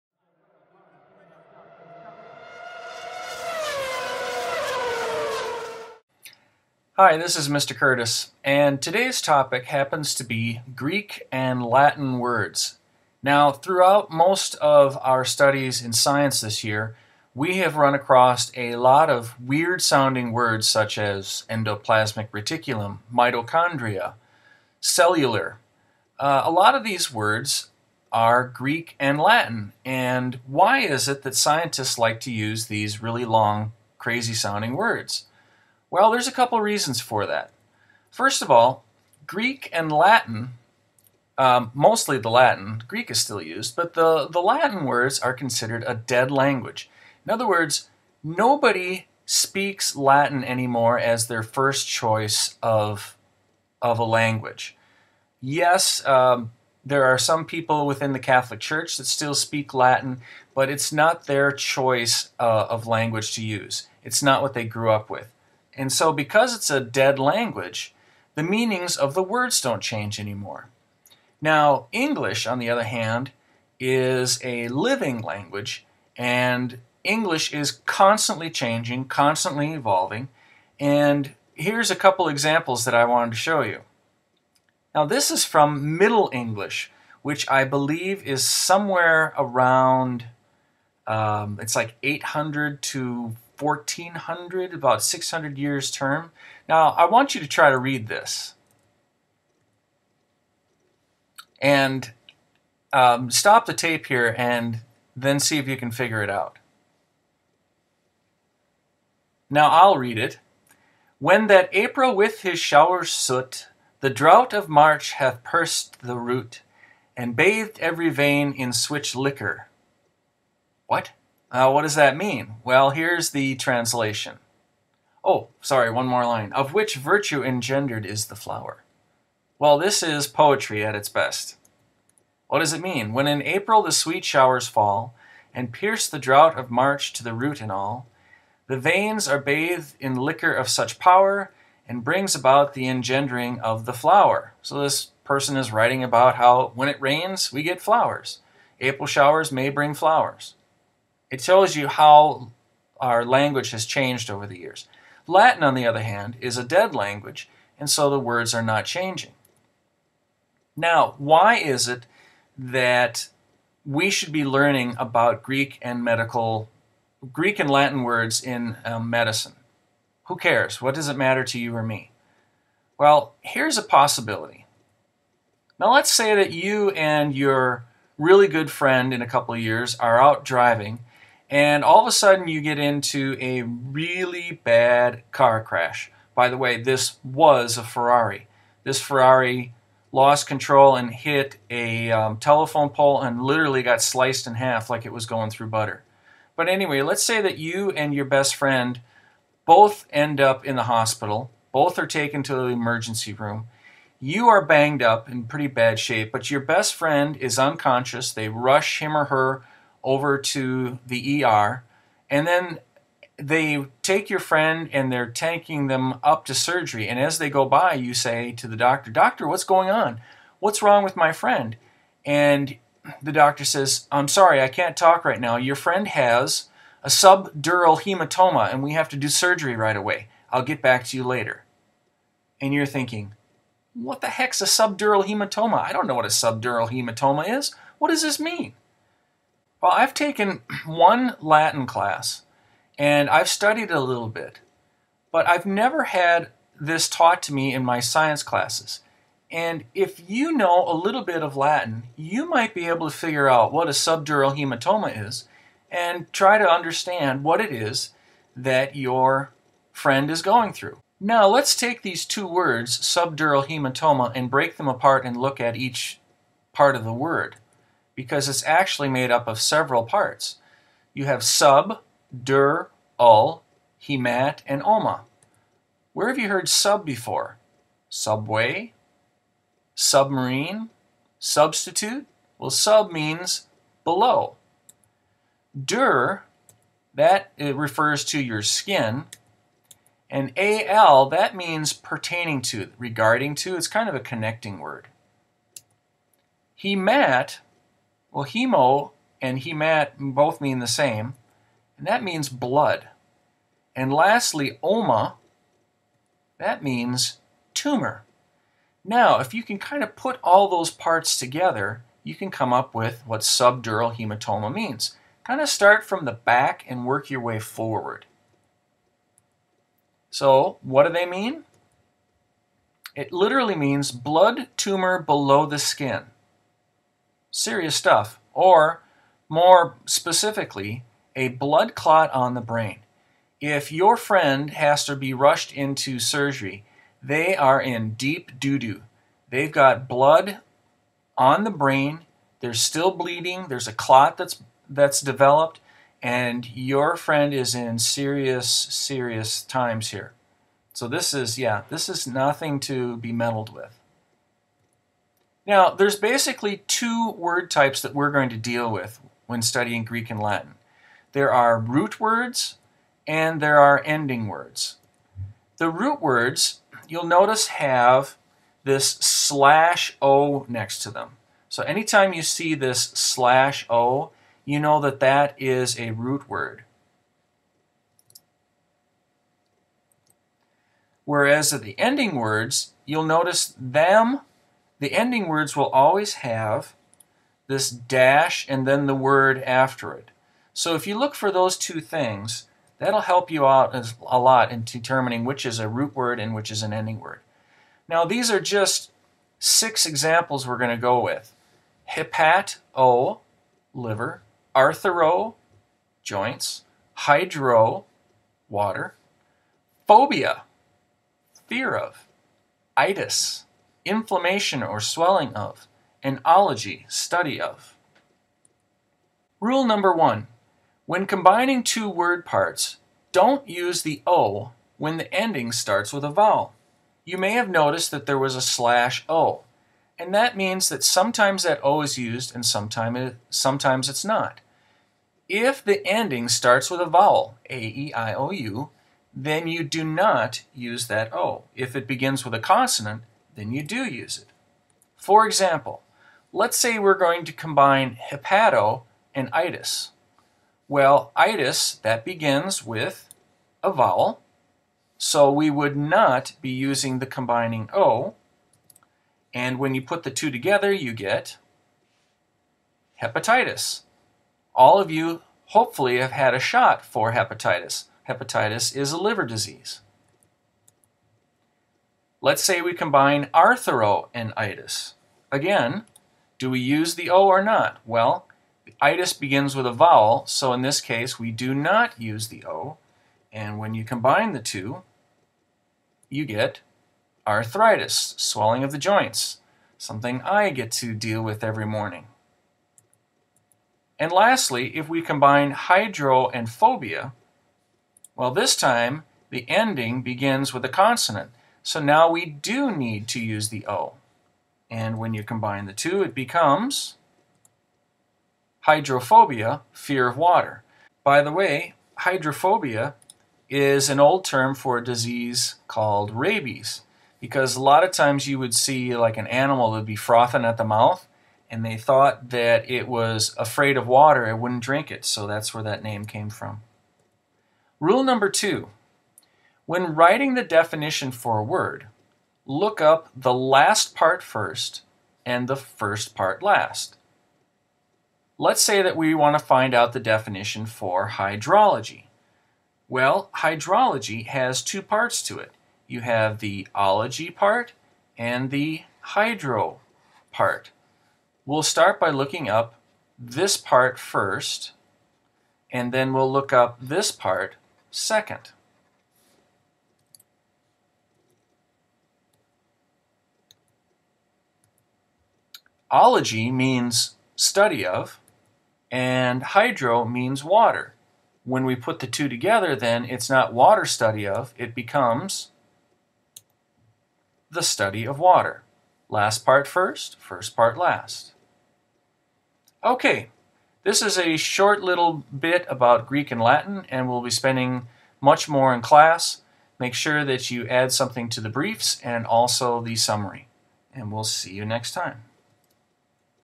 Latin
introduction to why scientists use latin